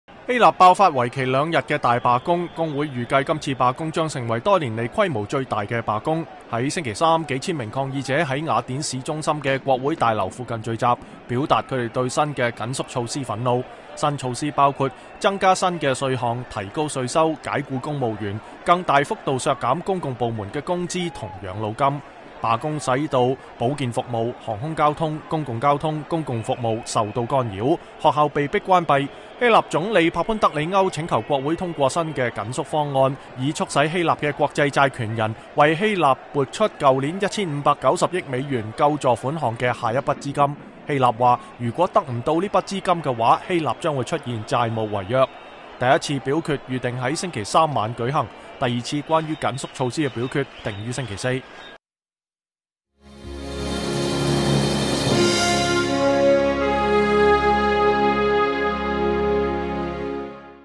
2011-10-19 美國之音視頻新聞: 希臘國會表決緊縮措施前爆發罷工
希臘爆發為期兩天的大罷工。工會預計﹐這次罷工將成為多年來規模最大的罷工。